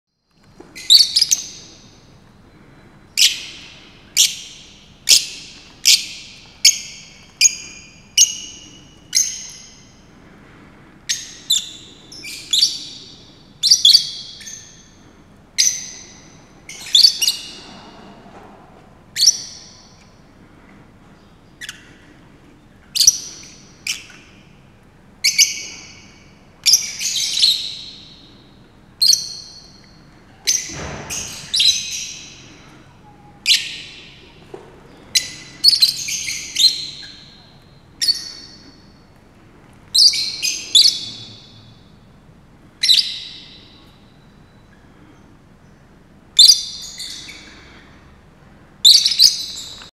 Fischer's lovebird (Agapornis fischeri) is singing a song
This lovebird was singing a little song and he was kinda dancing, while doing so. It was really lovely to watch.

Location: Saarbrücker Zoo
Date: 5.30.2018
Equipment: Nikon D3200 + AF-S NIKKOR 50mm f/1.8G